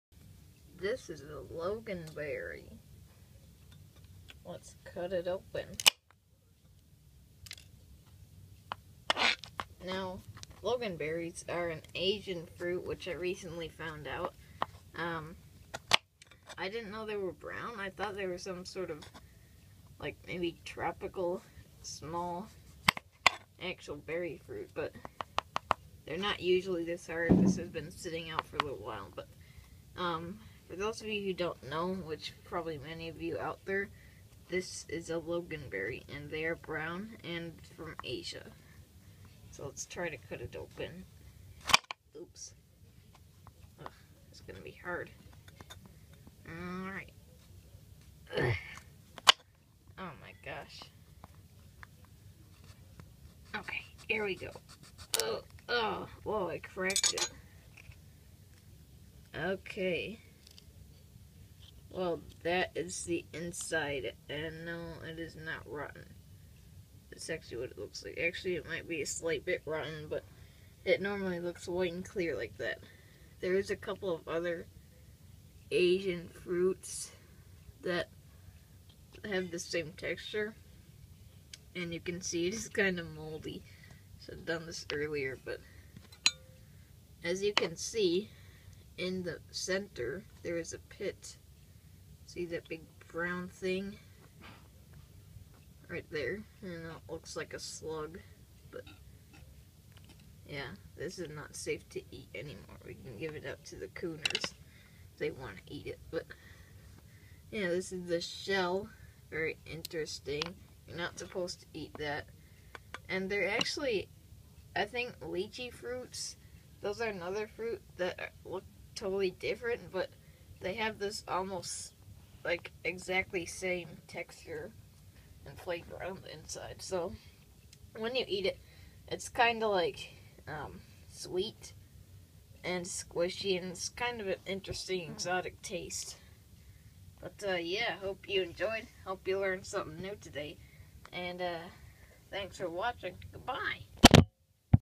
This is a Loganberry
Yalls today we are looking on the inside of a very stale loganberry. For those who have never seen one, here it is! TankQ for watching and THUBTHKRIBE!!